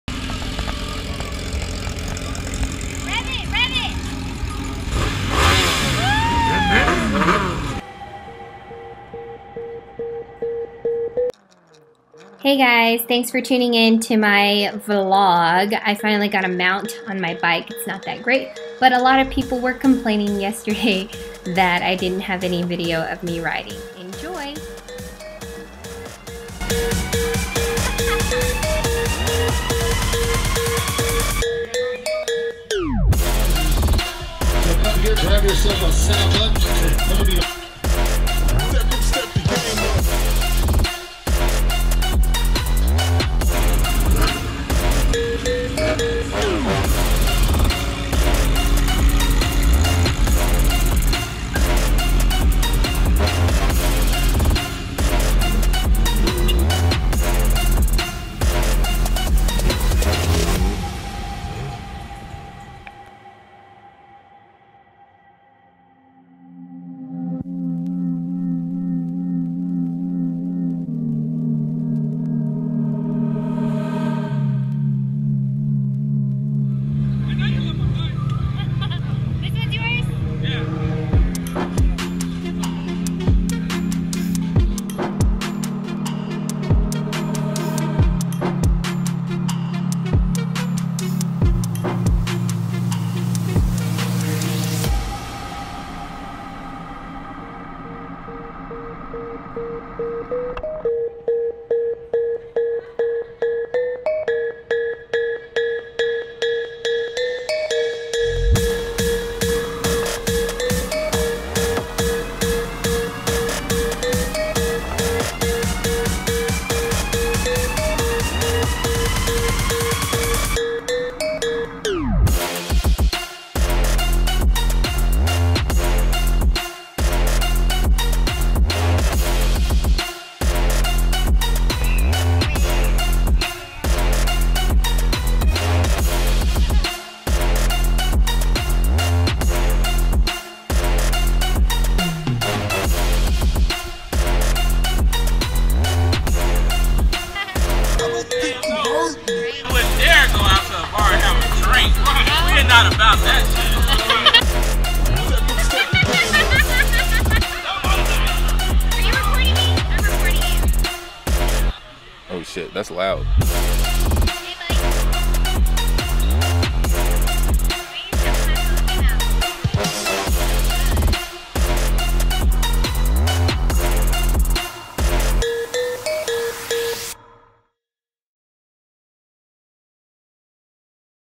TeeHeeNee Vlog 2: MotoGP Week
Hey guys! Finally got a mount -- but it's not working too well so I might switch to a helmet mount. Here's day 2 of festivities this week. We rode to Cycle Gear in Austin to meet up with some folks. Got some free swag, ate some free food. Then we went to Ducati, and then headed to Bombshells with our moto friends.

-Nee